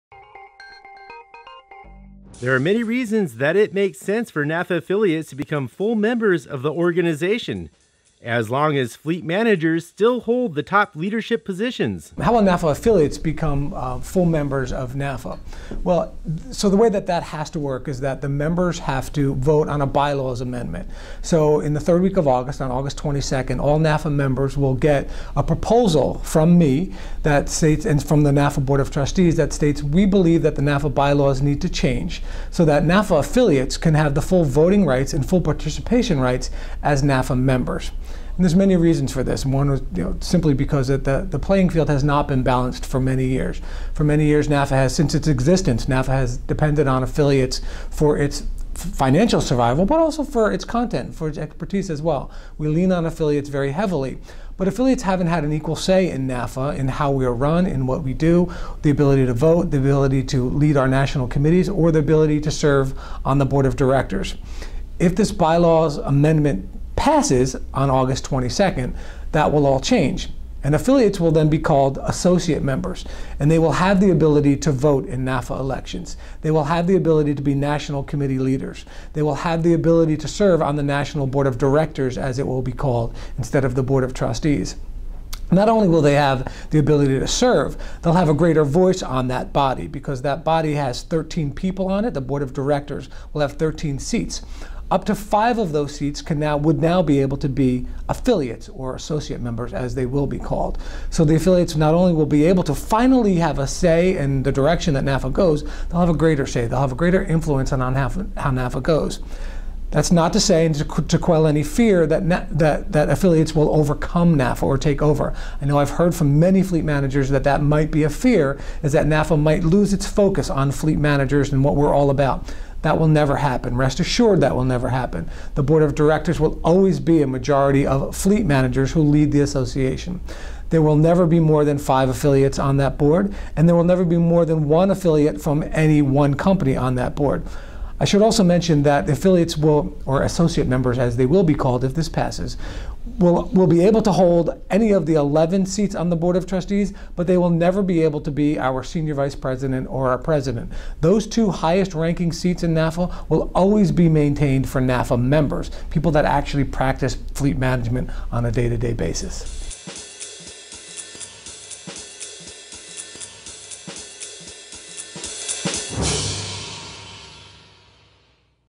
How (and Why) Should NAFA Affiliates Become Full Members? | PHIL RUSSO | Fleet Management Weekly
There are many reasons that it makes sense for NAFA affiliates to become full members of the organization - as long as fleet managers still hold the top leadership positions.¬¬¬

PHIL RUSSO
Chief Executive Officer
NAFA Fleet Management Association

160718FmwPRusso01